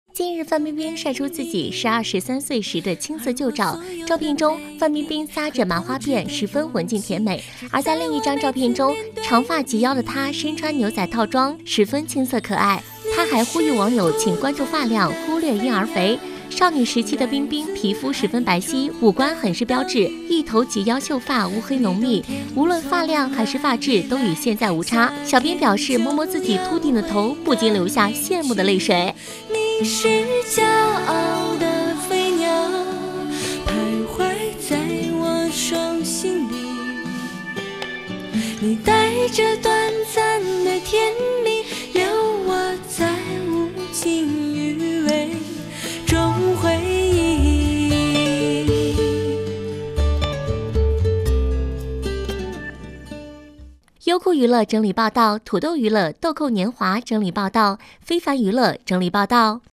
【2年前】范冰冰曬豆蔻年華青澀舊照 天生髮量驚人嬰兒肥可愛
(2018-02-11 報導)

范冰冰曬出自己12、13歲時的青澀舊照，長髮及腰的她身穿牛仔套裝，十分青澀可愛，她還呼吁網友“請關注發量，忽略嬰兒肥”。另一張照片中，范冰冰扎著麻花辮，十分文靜甜美